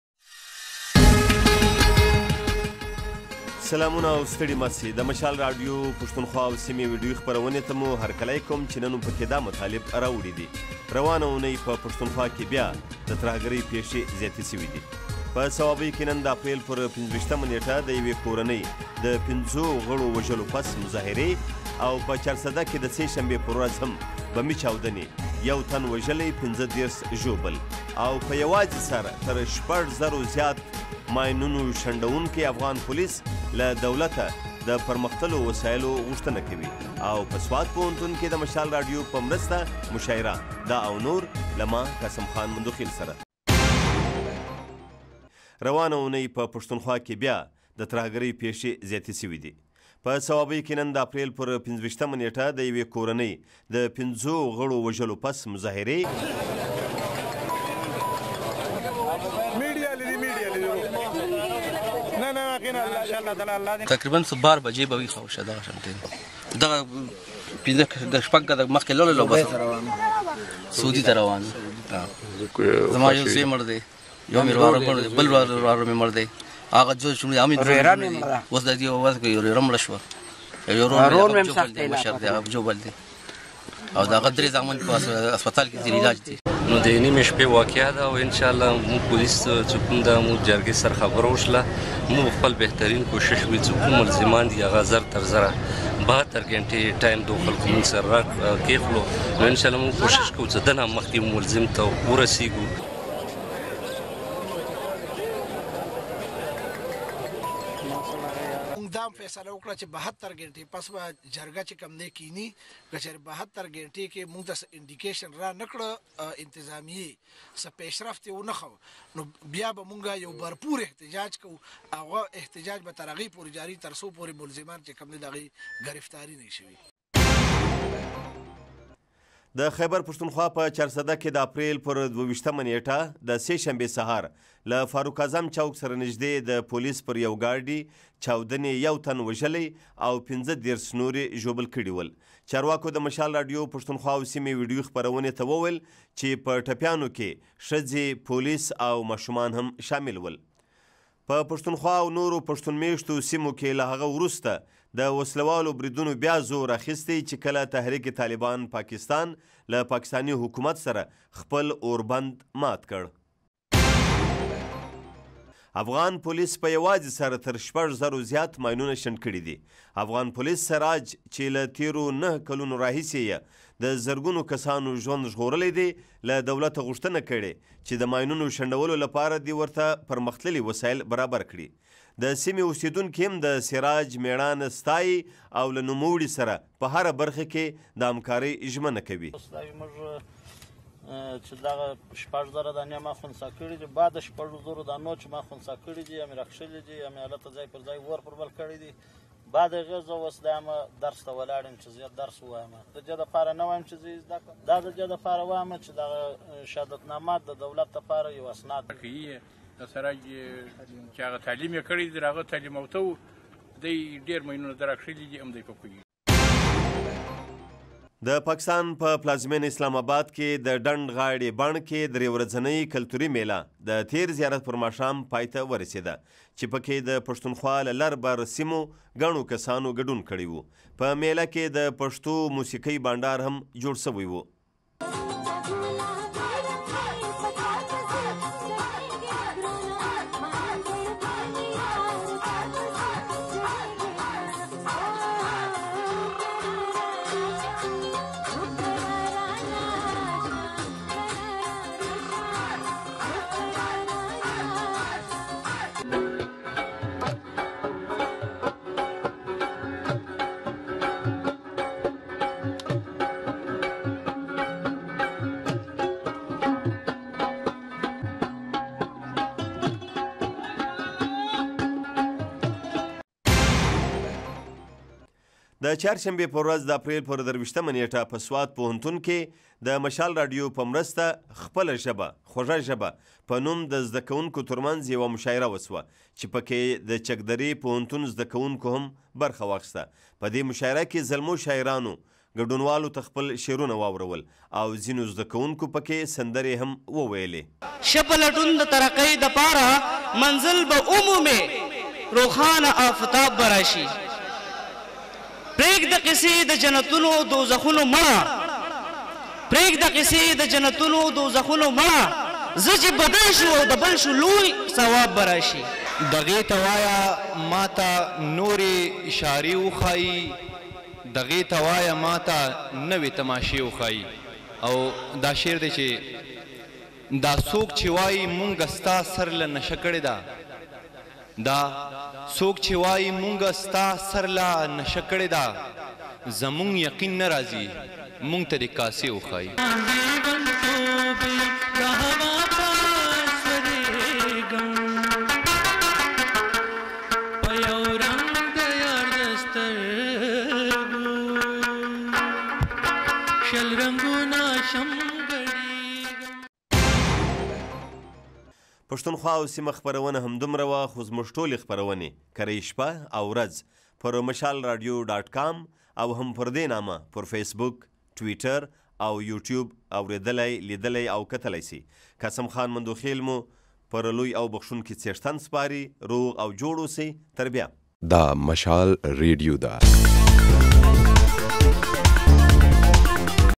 پښتونخوا او سیمه
د خېبرپښتونخوا په صوابۍ کې د یوې کورنۍ پنځه تنه غړي د وسله والو په یو برید کې وژل شوي. په چارسده کې د تېرې سېشنبې پر ورځ بمي چاودنې یو تن وژلی او ۳۵ نور ژوبل ول دا او نور د پښتونخوا او سيمې ويډیويي خپرونه کې.

 وړاندېکوونکی: قاسم خان مندوخېل